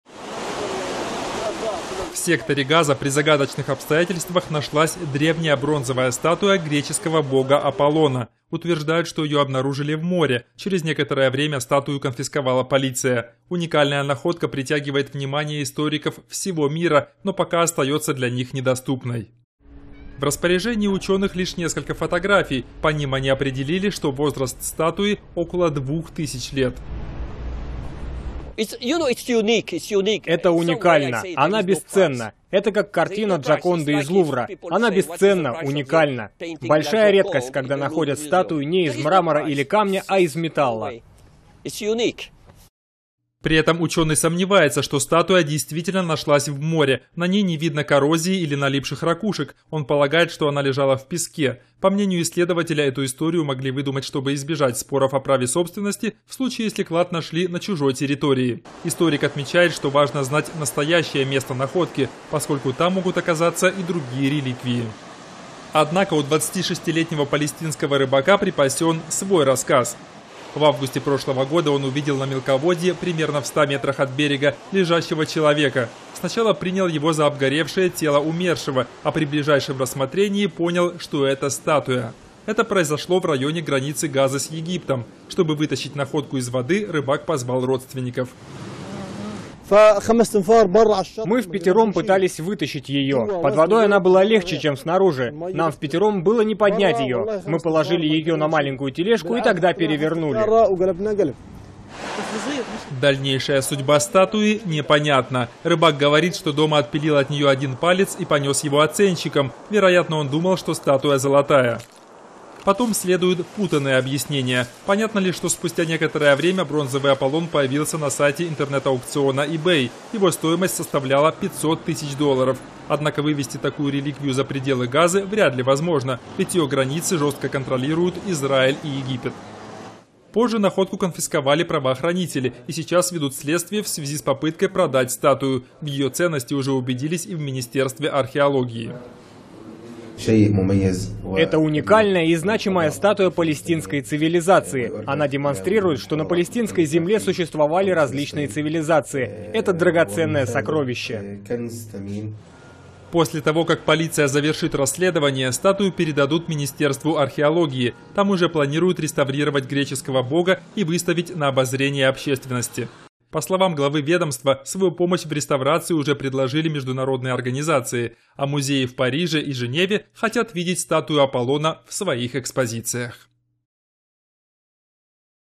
В Газе нашли загадочную бронзовую статую Аполлона (новости)
В Газе нашли загадочную бронзовую статую Аполлона. Загадка появления уникальной статуи Аполлона в секторе Газа так и остаётся нераскрытой. Однако у историков появилась надежда увидеть реликвию воочию. Власти обещают показать её миру.

В секторе Газа при загадочных обстоятельствах нашлась древняя бронзовая статуя греческого бога Аполлона. Утверждают, что её обнаружили в море. Через некоторое время статую конфисковала полиция. Уникальная находка притягивает внимание историков всего мира, но пока остаётся для них недоступной.

В распоряжении учёных -- лишь несколько фотографий. По ним они определили, что возраст статуи -- около двух тысяч лет.

[Жан-Мишель де Таррагон, историк]:
«Это -- уникально. Она - бесценна. Это -- как картина Джоконды из Лувра. Она бесценна, уникальна. Большая редкость, когда находят статую не из мрамора или камня, а из металла».

При этом учёный сомневается, что статуя действительно нашлась в море. На ней не видно коррозии или налипших ракушек. Он полагает, что она лежала в песке. По мнению исследователя, эту историю могли выдумать, чтобы избежать споров о праве собственности в случае, если клад нашли на чужой территории. Историк отмечает, что важно знать настоящее место находки, поскольку там могут оказаться и другие реликвии.

Однако у 26-летнего палестинского рыбака припасен свой рассказ. В августе прошлого года он увидел на мелководье, примерно в ста метрах от берега, лежащего человека. Сначала принял его за обгоревшее тело умершего, а при ближайшем рассмотрении понял, что это -- статуя. Это произошло в районе границы Газы с Египтом. Чтобы вытащить находку из воды, рыбак позвал родственников.

[Джудат Храб, рыбак]:
«Мы впятером пытались вытащить её. Под водой она была легче, чем снаружи. Нам впятером было не поднять её. Мы положили её на маленькую тележку и тогда перевернули».

Дальнейшая судьба статуи непонятна. Рыбак говорит, что дома отпилил от неё один палец и понёс его оценщикам. Вероятно, он думал, что статуя золотая. Потом следуют путаные объяснения. Понятно лишь, что спустя некоторое время бронзовый Аполлон появился на сайте интернет-аукциона eBay. Его стоимость составляла 500 тысяч долларов. Однако вывезти такую реликвию за пределы Газы вряд ли возможно, ведь её границы жёстко контролируют Израиль и Египет.

Позже находку конфисковали правоохранители и сейчас ведут следствие в связи с попыткой продать статую. В её ценности уже убедились и в Министерстве археологии.